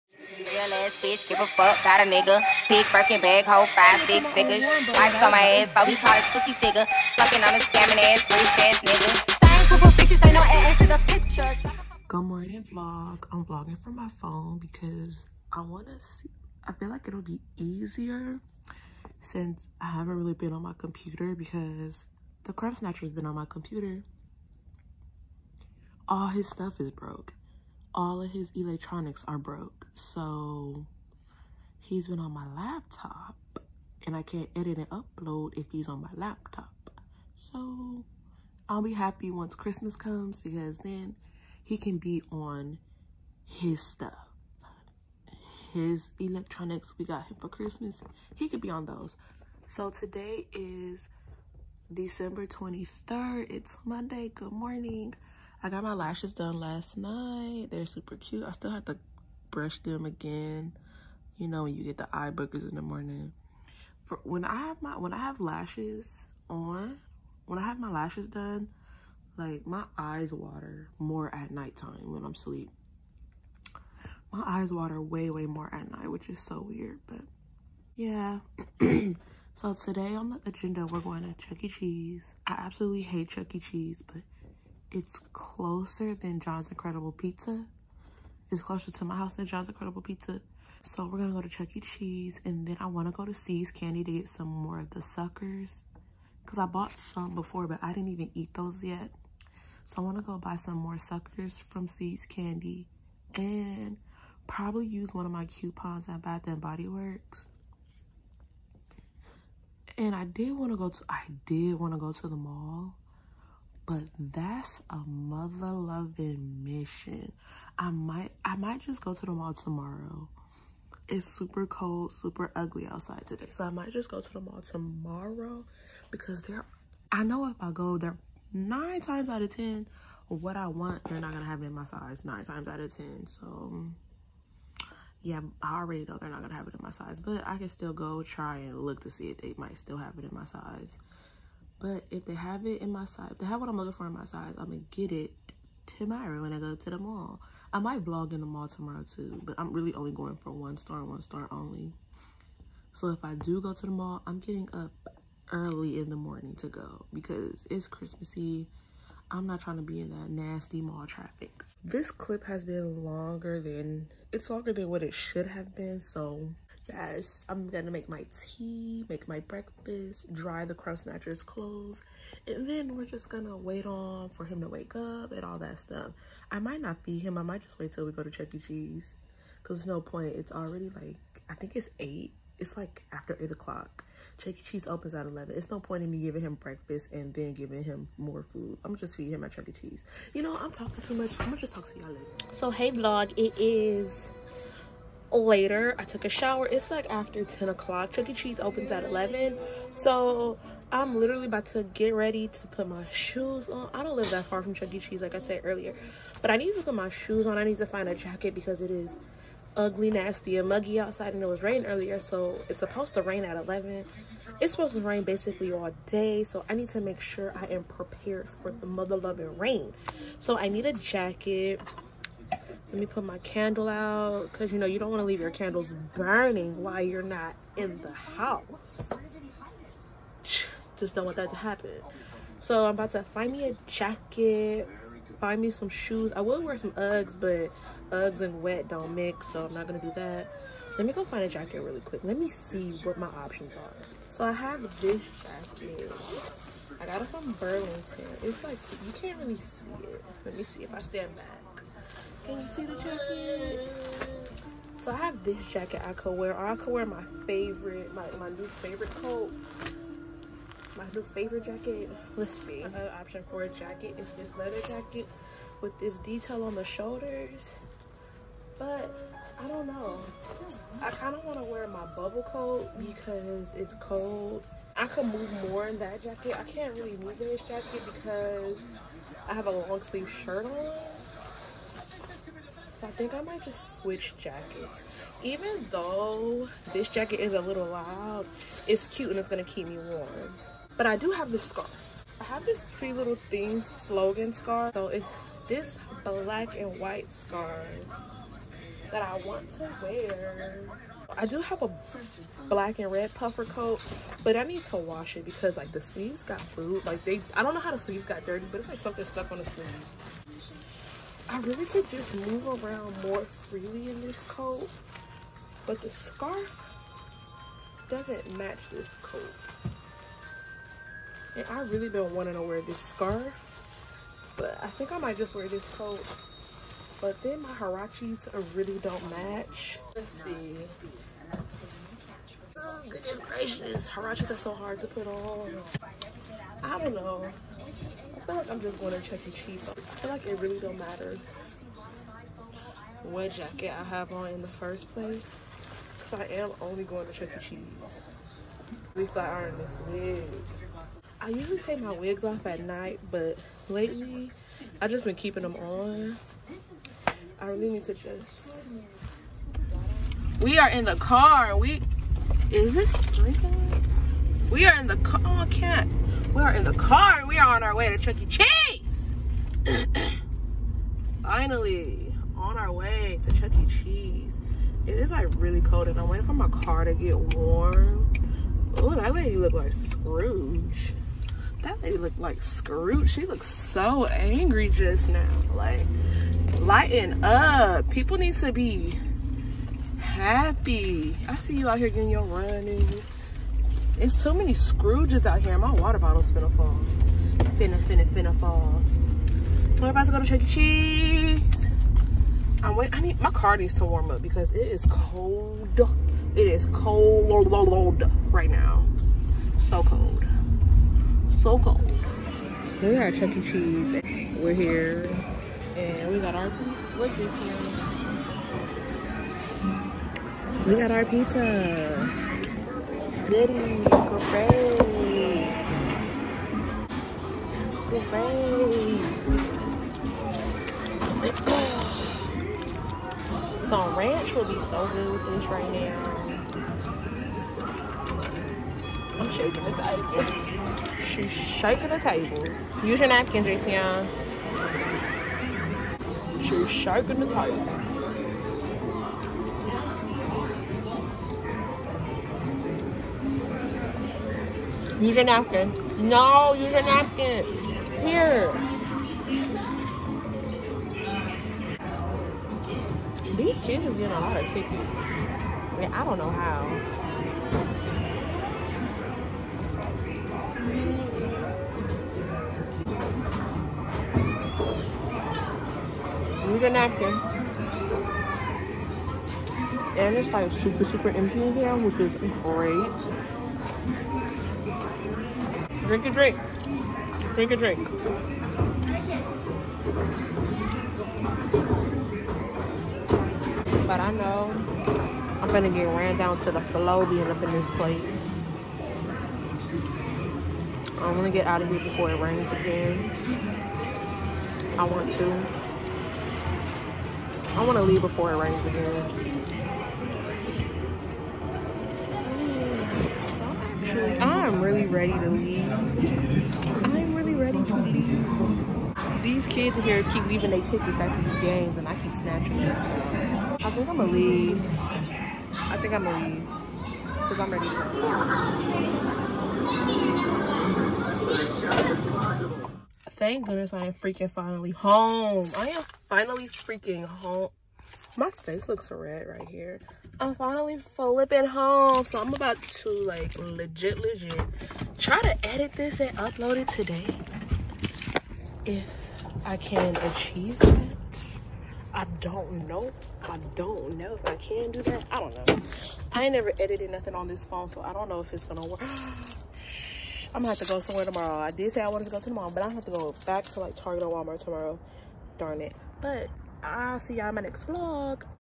Vlog - I need A Jacket To Wear Go Chuck E Cheese
Took the crumb snacher to Chuck E. Cheese and I couldn’t decided on what jacket to wear
Plus these kids kept leaving they tickets at the games
Swiper done swiped the tickets on up